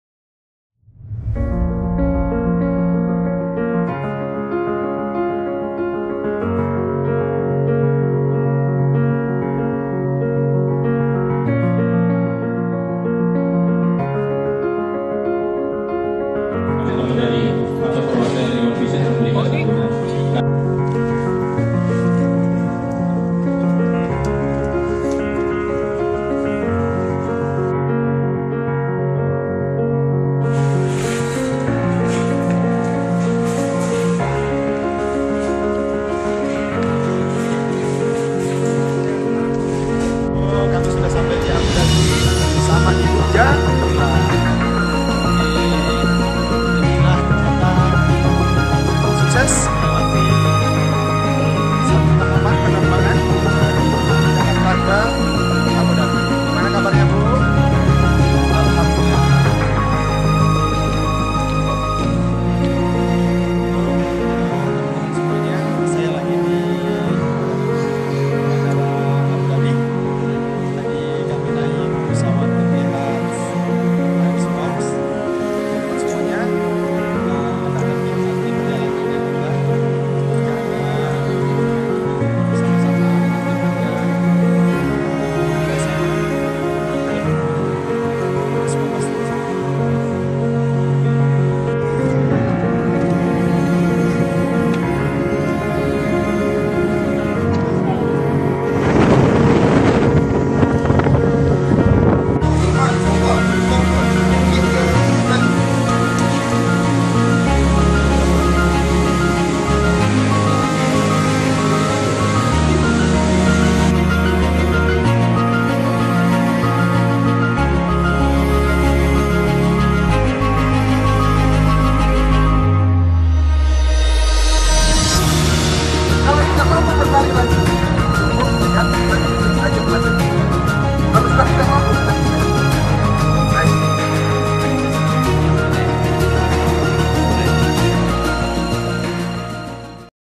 VLOG #4 Fly with Etihad Airways Abu Dhabi
Penerbangan Saat Berangkat Umrah From Pekanbaru fly to Jakarta Lalu Fly Menuju Abu Dhabi dan Fly Menuju King Abdul Aziz Jeddah... Perjalanan pertama Umrah dalam Hidup...